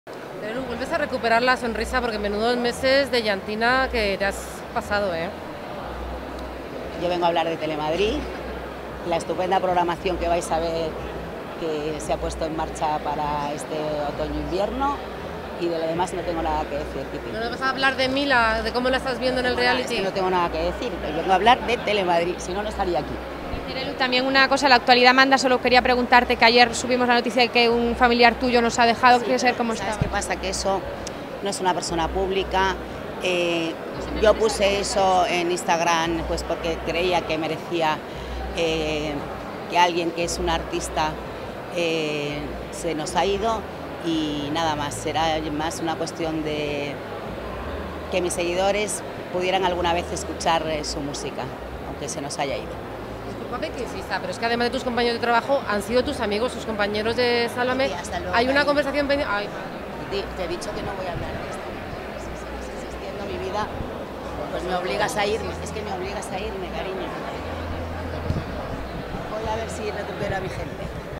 Terelu Campos planta a la prensa tras una pregunta | Qué me dices
Terelu Campos no quiere hablar ni de Sálvame, ni de Mila Ximénez, ni de sus excompañeros. A su llegada al acto de presentación de la programación de TeleMadrid, Terelu, atendió a la prensa congregada. Tras unos minutos contestando se le preguntó por sus "últimos meses de llantina" en clara alusión a su situación en Tele5. La colaboradora argumentó que no iba a hablar del asunto. La hija de María Teresa Campos dijo que tocaba hablar de TeleMadrid. Cuando la periodista insistió Terelu Campos dió por terminada la rueda de prensa.